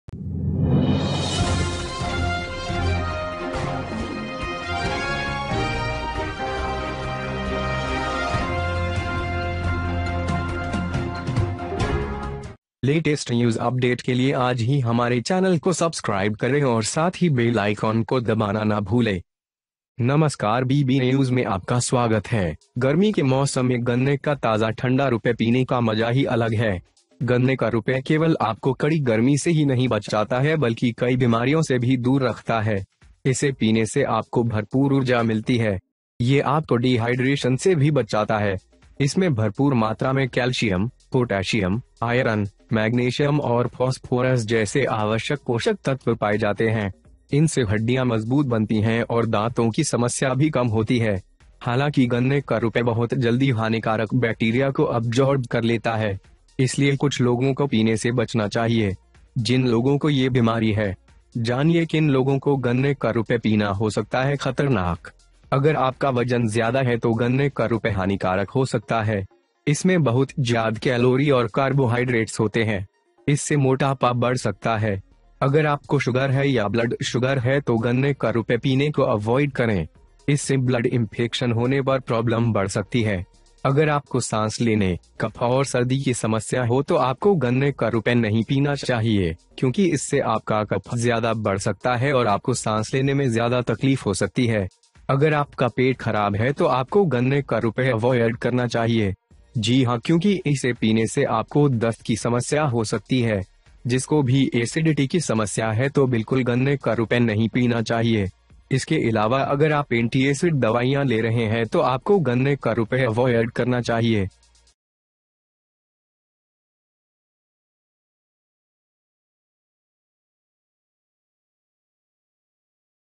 गन्ने का रस पिने वाले हो जाये सावधान, भूलकर भी ये लोग न पीये गन्ने का रस...
It is only fun to enjoy fresh cold juice of sugarcane during the summer season. Sugarcane juice does not only save you from the heat but also keeps away from many diseases. Drinking it gives you plenty of energy. It also protects you from dehydration.

It contains essential nutrients such as calcium, potassium, iron, magnesium and phosphorus in abundance. Bones are strengthened by these and teeth problems are also less. Although sugar cane juice very quickly destroys harmful bacteria. Therefore some people should avoid drinking. People who have this disease. Know which people can drink sugarcane juice is dangerous

Sugarcane juice can be harmful if your weight is high. There are a lot of calories and carbohydrates in it. This can lead to obesity. If you have sugar or blood sugar, then avoiding sugarcane juice. Problems with blood infection can increase.

If you have breathing problems, phlegm and cold, then you should not drink sugarcane juice because it can increase your phlegm and you may have more trouble breathing.

If your stomach is bad then you should absorb sugarcane juice. Yes, because drinking it can cause you diarrhea. Anyone who has acidity problems should not drink the sugarcane juice completely. Apart from this, if you are taking antipsid medicines, you should absorb sugarcane juice.

BB NEWS!!